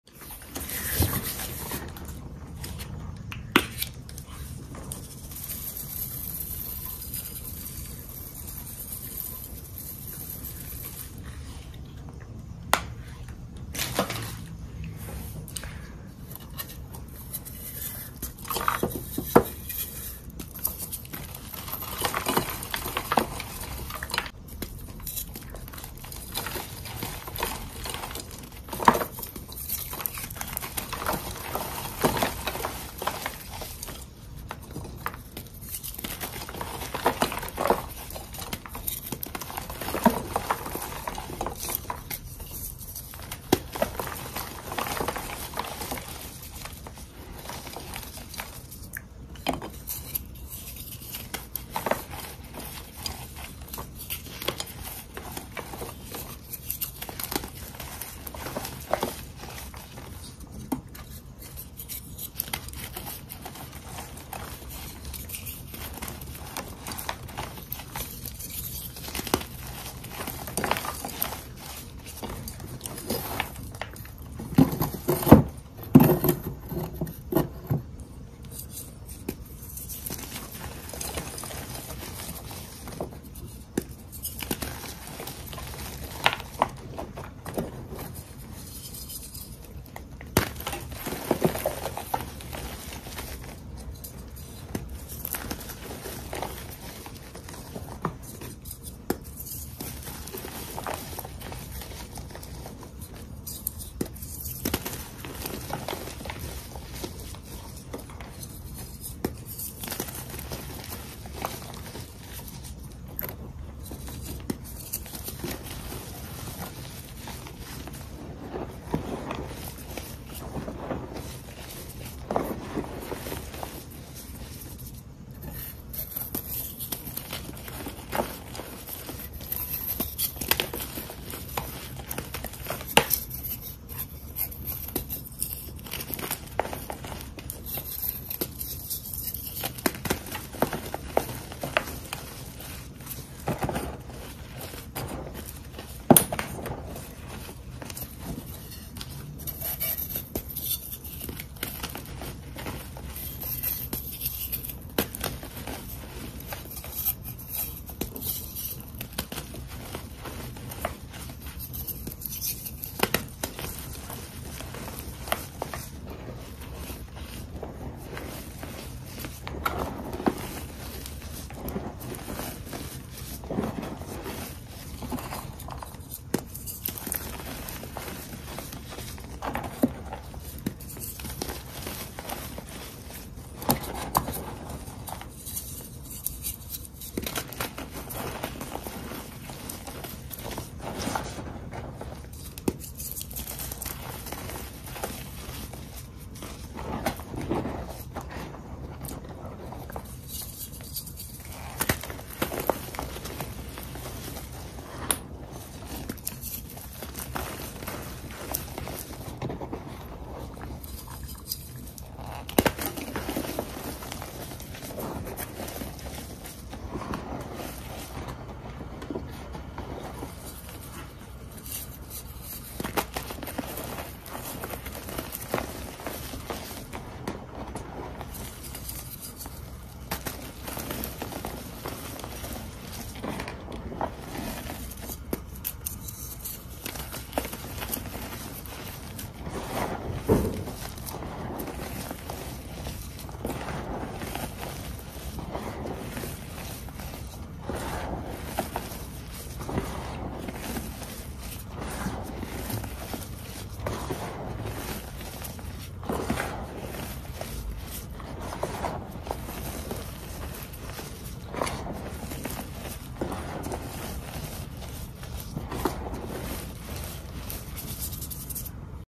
PJ Glitter Mix Reforms ♥️♥️ Valentines Day | Gym Chalk | ASMR | Sleep Aid | Oddly Satisfying
Plain Jane red glitter mix (Valentine’s Day edition)
Hearts & Petals